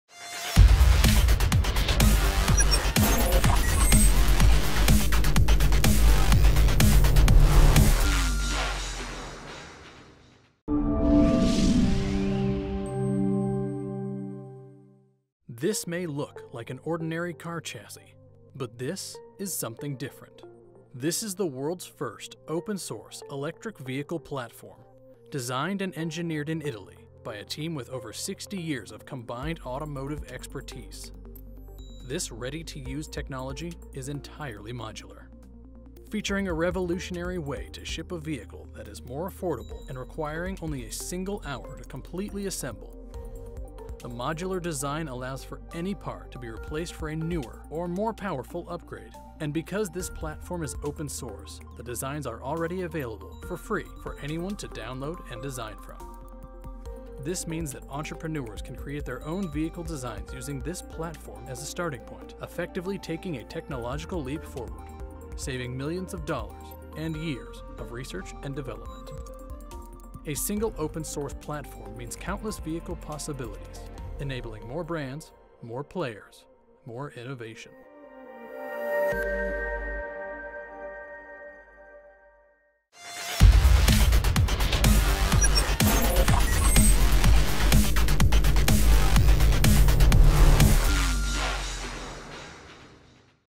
OSVehicle Open Source Modular Platform for Electric Vehicle assembly in 1 hour
OSVehicle
Open Source Modular Platform for Electric Vehicle assembly in 1 hour